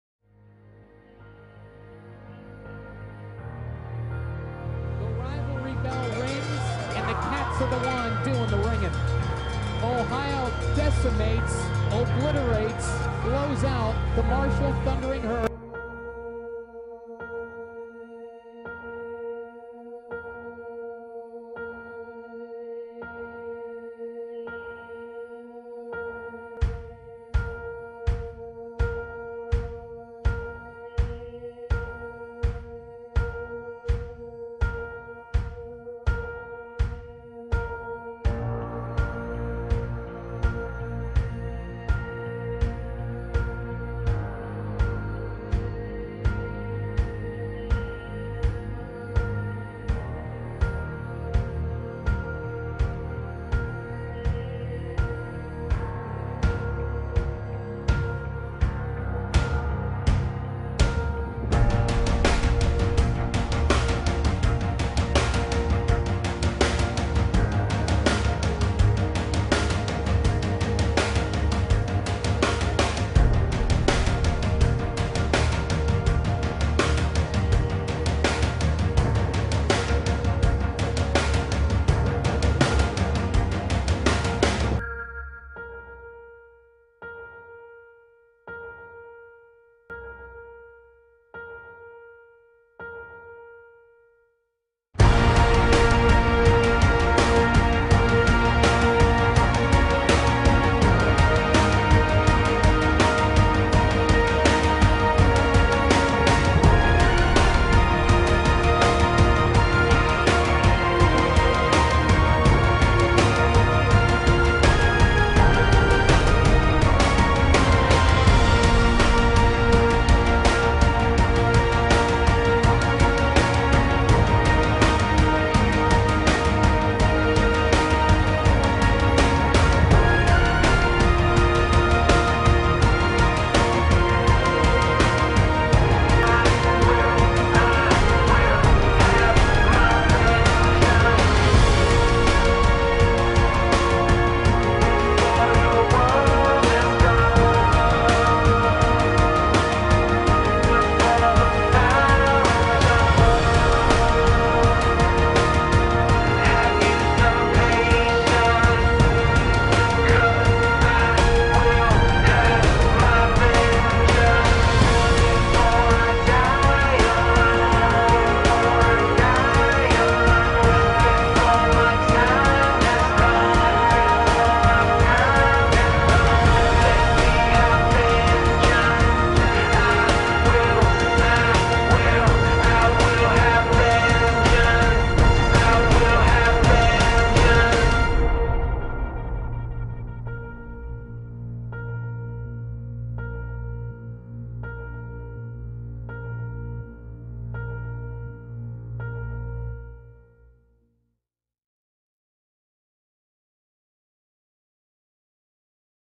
Marshall 2012 Battle for the Bell Hype: Vengeance
The Thundering Herd seeks revenge for the 44-7 loss to Ohio in 2011.